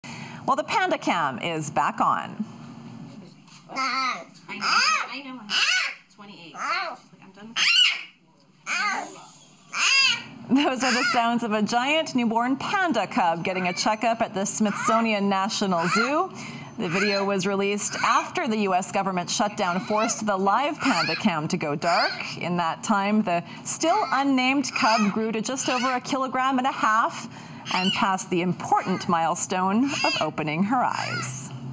BT Vancouver: Amazing Newborn Panda Sounds
We do love our baby animal videos here on BT, but the footage you're about to see may be the most adorable yet. Take a look.